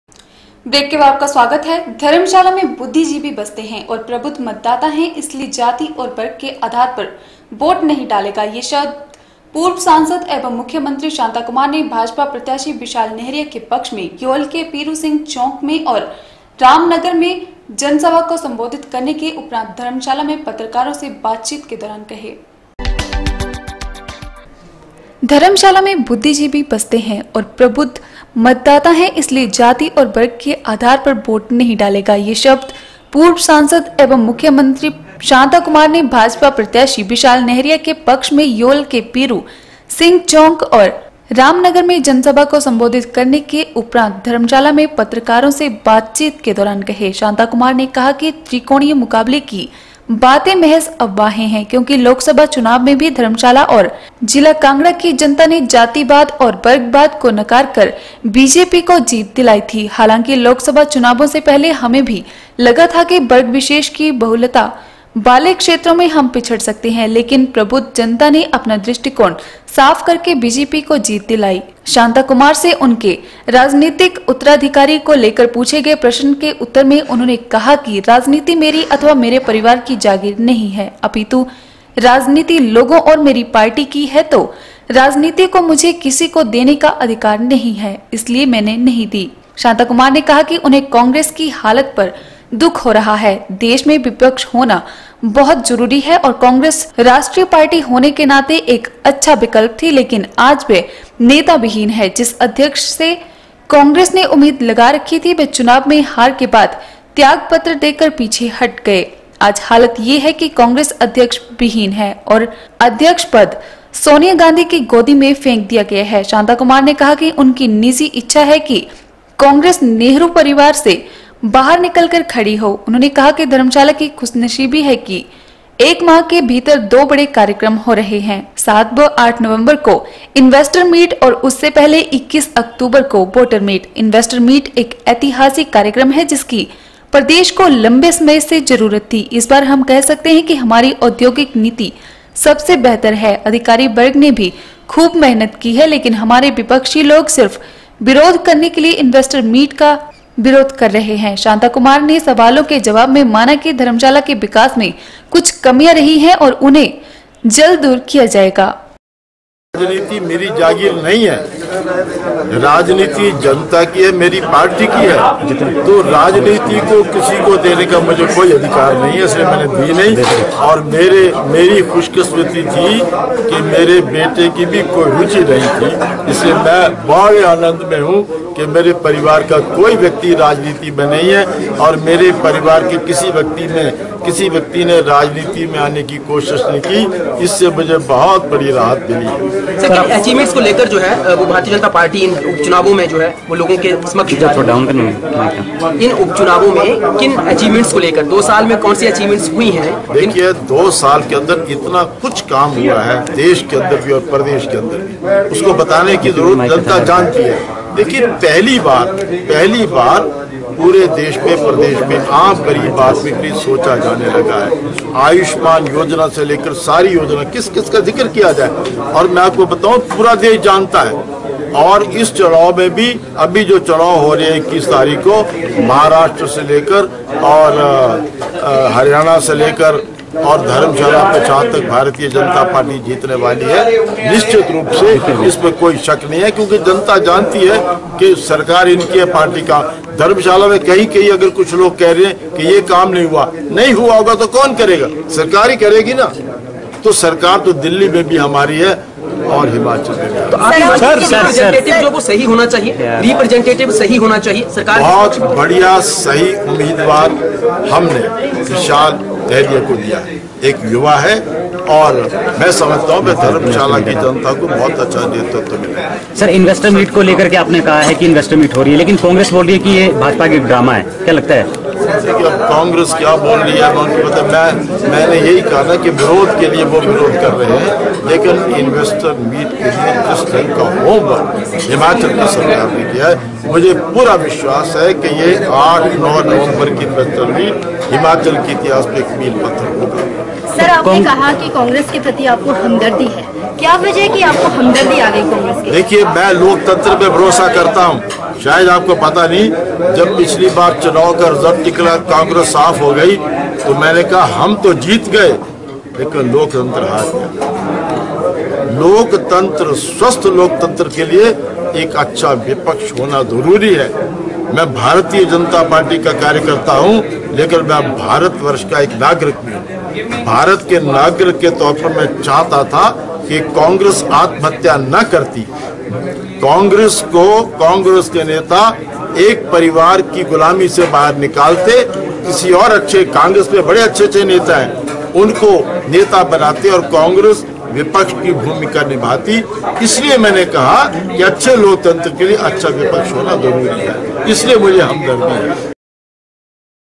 15 OCT N 8 जाति और वर्ग के आधार पर वोट नहीं डालेगा
: धर्मशाला में बुद्धिजीवी बसते हैं और प्रबुद्ध मतदाता हैं इसलिए जाति और वर्ग के आधार पर वोट नहीं डालेगा। यह शब्द पूर्व सांसद एवं मुख्यमंत्री शांता कुमार ने भाजपा प्रत्याशी विशाल नेहरिया के पक्ष में योल के पीरु सिंह चौक और राम नगर में जनसभा को संबोधित करने के उपरान्त धर्मशाला में पत्रकारों से बातचीत के दौरान कहे। शांता कुमार ने कहा कि त्रिकोणीय मुकाबले की बातें महज अफवाहें हैं। क्योंकि लोकसभा चुनाव में भी धर्मशाला और जिला कांगड़ा की जनता ने जातिवाद और वर्गवाद को नकारकर बीजेपी को जीत दिलाई थी। हालांकि लोकसभा चुनाव से पहले हमें भी लगा था कि वर्ग विशेष की बहुलता वाले क्षेत्रों में हम पिछड़ सकते हैं लेकिन प्रबुद्ध जनता ने अपना दृष्टिकोण साफ करके बीजेपी को जीत दिलाई ।